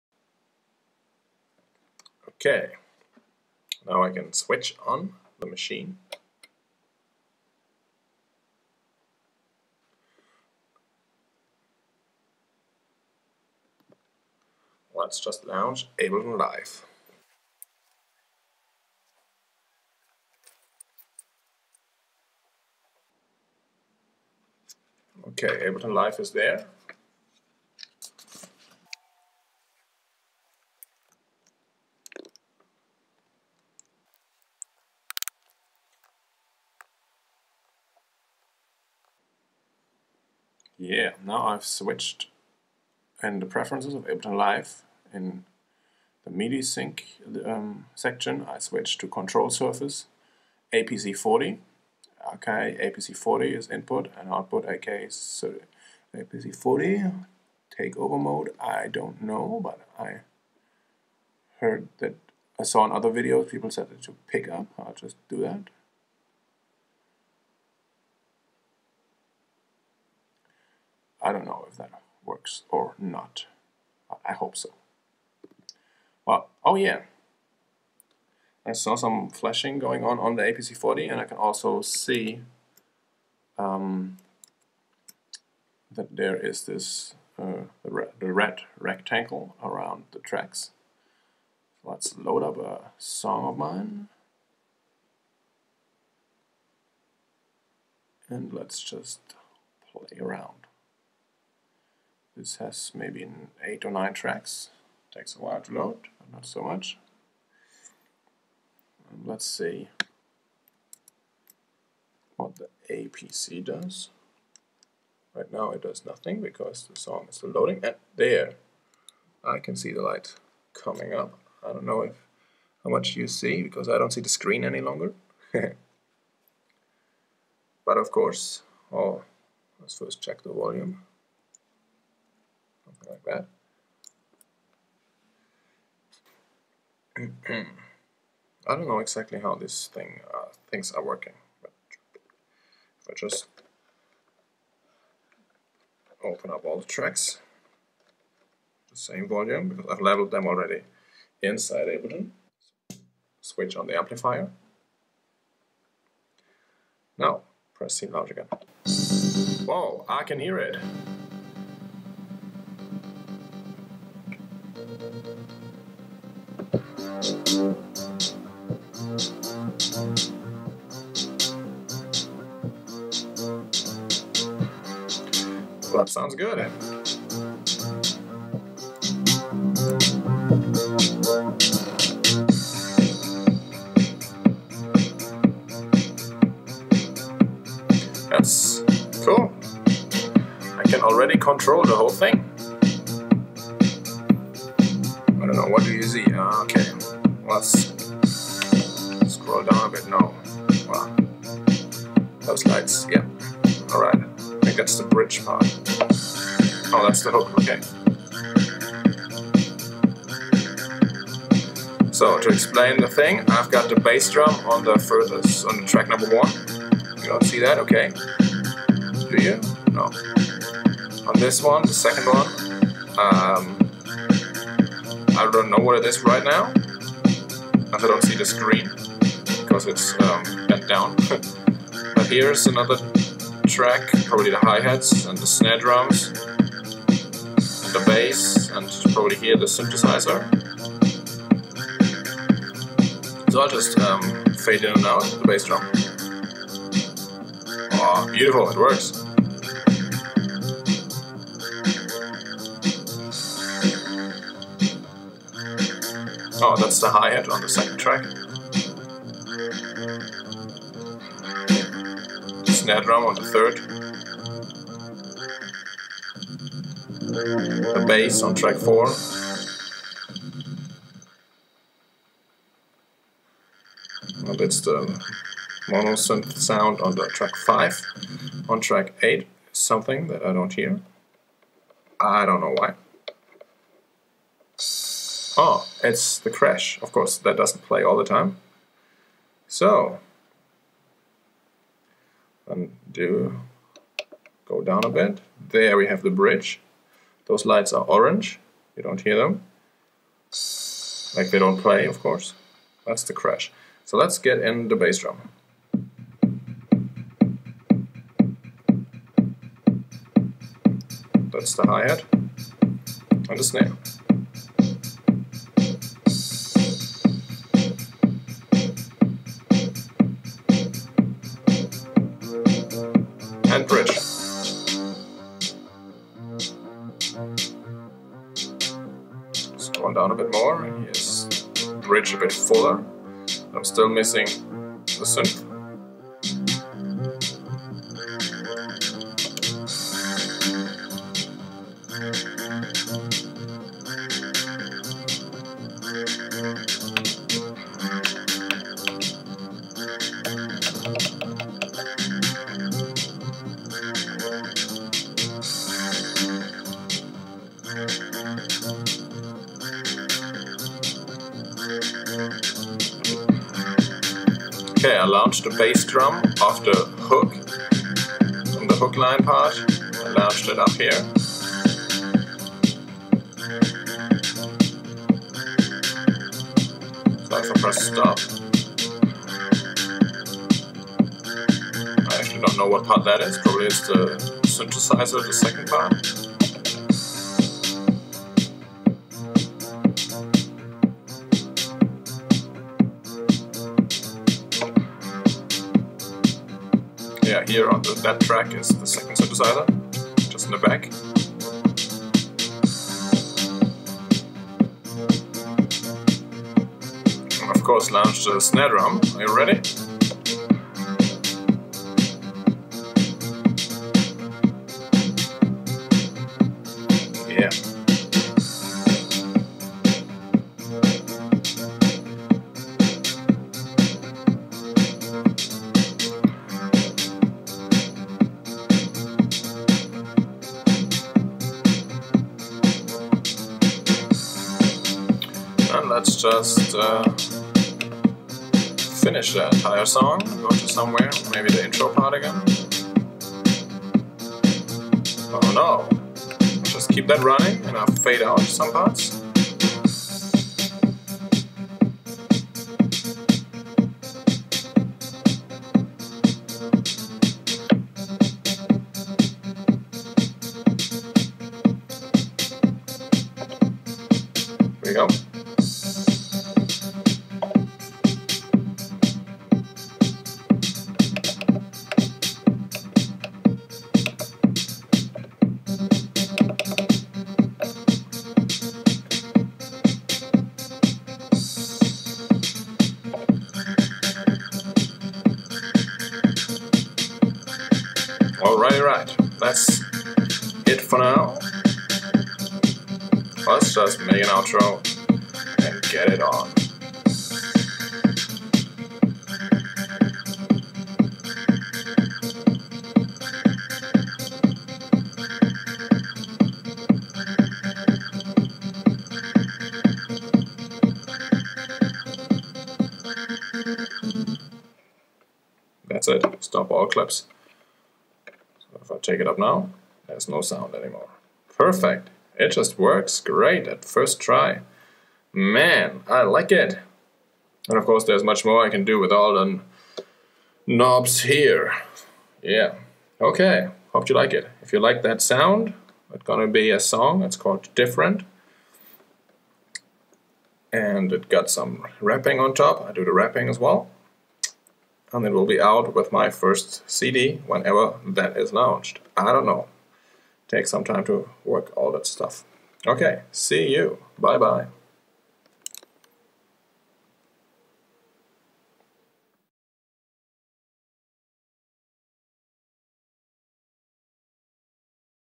RXL turns on the APC40 for the first time.
Watch as I turn on the APC40 for the first time with Live 7. If you like the song here's my